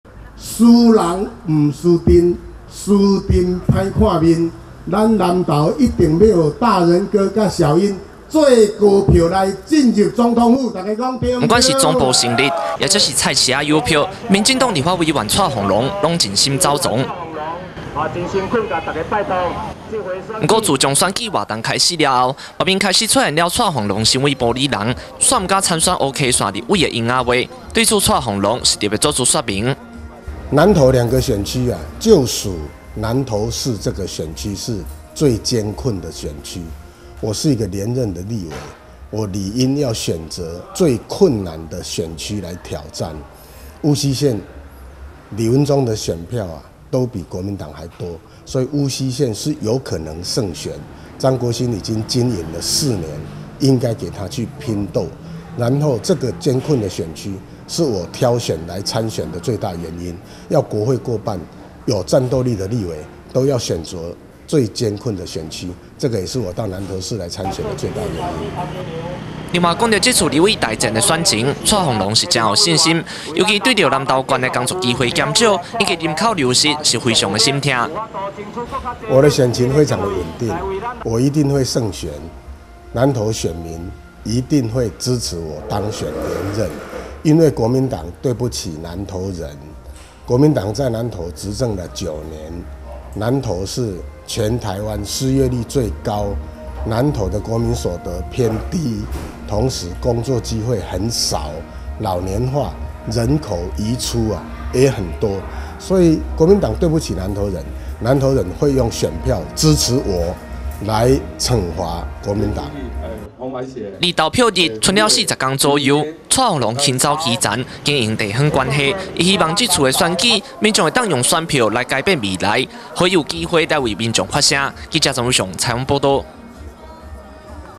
南投新聞-蔡煌瑯破除逃避烏溪線謠言
自從選舉活動開始之後，外面開始出現"蔡煌瑯身為埔里人，卻不敢參選烏溪線立委"的謠言，對此，立法委員蔡煌瑯就強調，身為連任的立委，應當要選擇困難的選區，加上張國鑫已經在烏溪線經營了好幾年的時間，所以這次才會決定參選濁水溪線的立委。